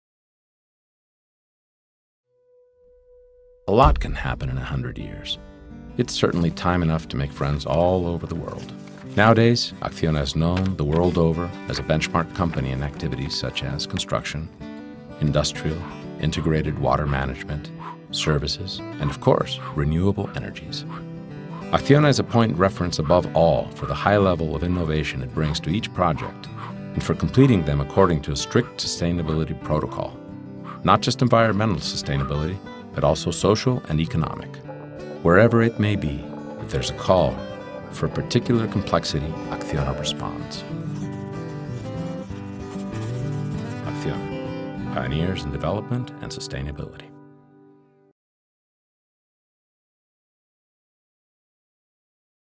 Around the world | Annual Report 2014 | ACCIONA
Learn more about ACCIONA's global activity in 2014 and its continual commitment to sustainable environmental, social, and economic development. ACCIONA has always been a pioneer in development and sustainability, applying a high level of innovation to each of its projects. Nowadays, ACCIONA is known the world over as a benchmark company in activities such as Construction, Industrial, Integrated Water Management, Services, and of course, Renewable Energies. Wherever it may be, if there is a call for particular complexity, ACCIONA responds.